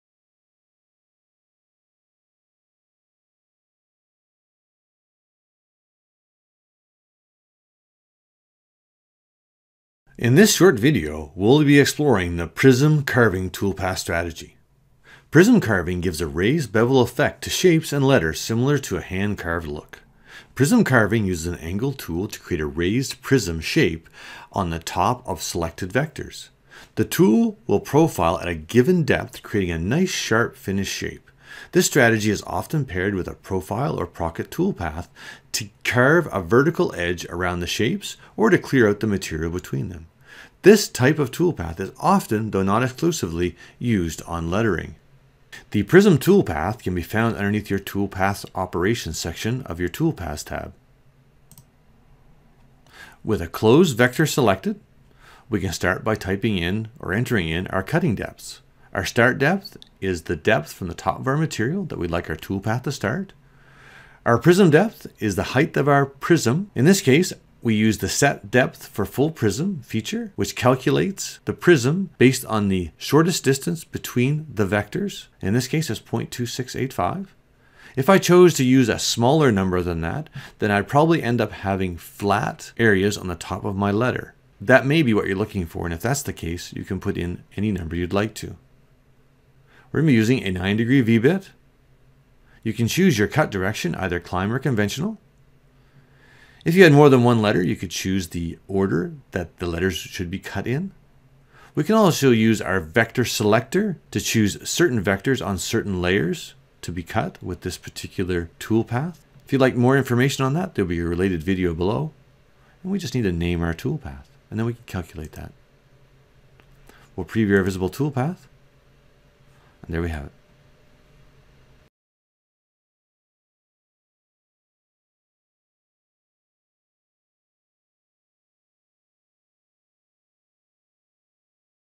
How to use the prism toolpath in Vectric software | 2.5D Toolpaths | V12 Tutorials
In this short video, we will examine the use of the prism toolpath and how to create raised prism-style lettering.

BOOKMARKS:
~ Cutting Depts - 00:55

To download the files used in this tutorial, view related videos or take advantage of our bookmarking system within the videos please head over to the support section of our website here: